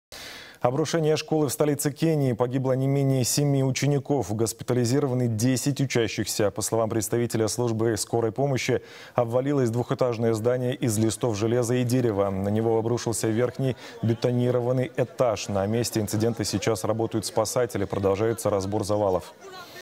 В Найроби обрушилась школа. Погибли дети
Обрушение школы в столице Кении - погибло не менее семи учеников. Госпитализированы 10 учащихся. По словам представителя службы скорой помощи, обвалилось двухэтажное здание из листов железа и дерева. На него обрушился верхний бетонированный этаж. На месте инцидента сейчас работают спасатели. Продолжается разбор завалов.